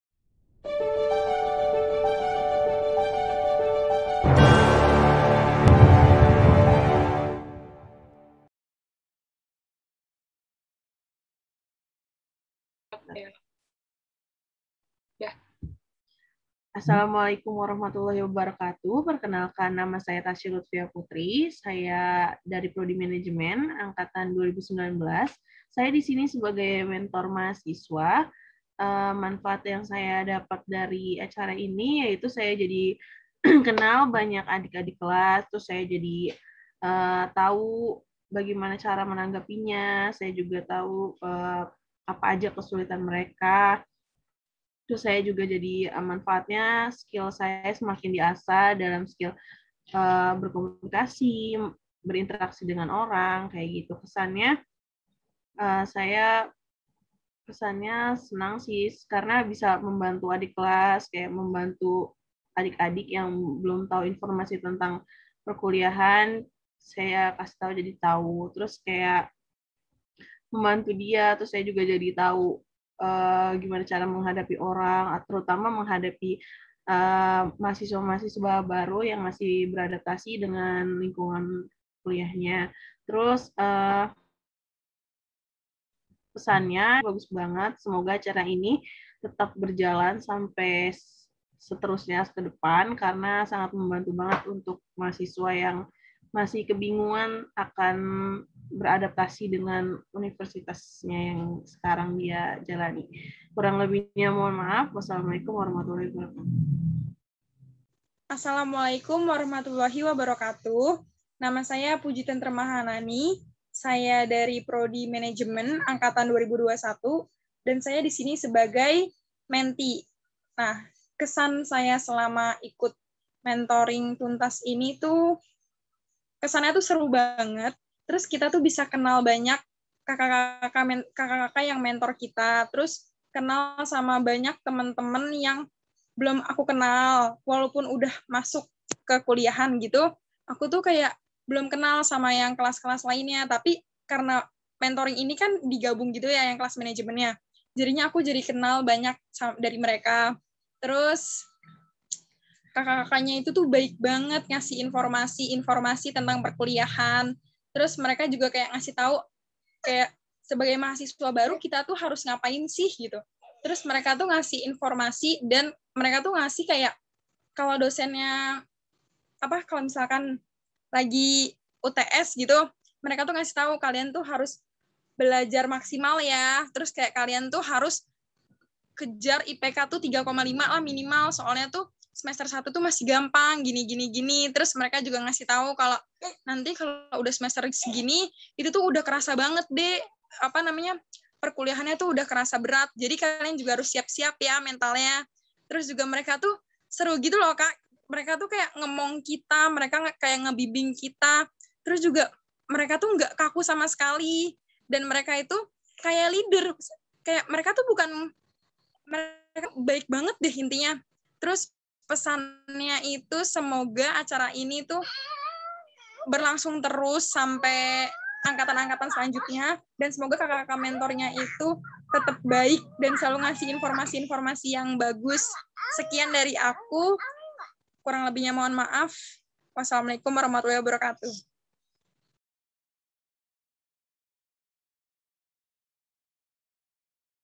Testimoni Program Mentoring bagi Mahasiswa Baru - Fakultas Ekonomi dan Bisnis - Prodi Manajemen
Video ini berisi testimoni mentor dan mentee dalam program mentoring bagi Mahasiswa Baru Universitas YARSI - Fakultas Ekonomi dan Bisnis - Prodi Manajemen